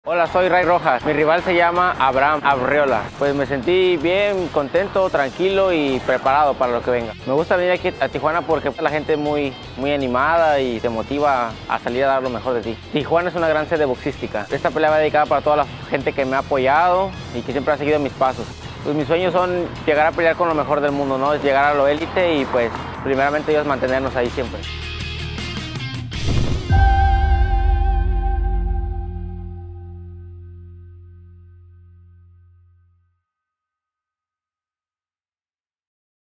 Ray Rojas en Tijuana
Se siente preparado para lo que venga además está entrenando con un gran entrenador del momento Isaac Cruz Sr padre de Pitbull Cruz